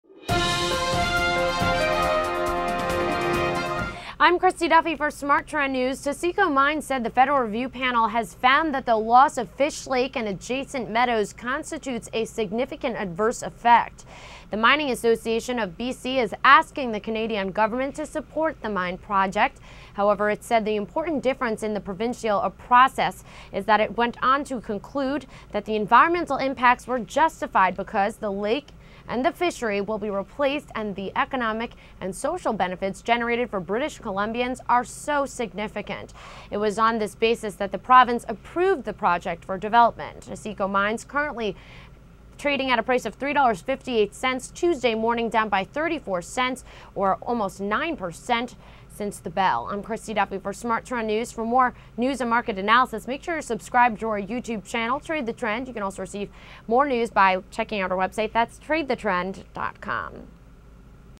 Taseko Mines Drops More Than 8.5%, Independent Panel Finds Adverse Effects From Proposed Project
Taseko Mines (TGB) said the Federal Review Panel has found that the loss of Fish Lake and adjacent meadows constitutes a significant adverse effect.

he Mining Association of B.C. is asking the Canadian government to support the mine project.

However, it said the important difference in the Provincial process is that it went on to conclude that the environmental impacts were justified because the lake and the fishery will be replaced and the economic and social benefits generated for British Columbians are so significant. It was on this basis that the Province approved the Project for development.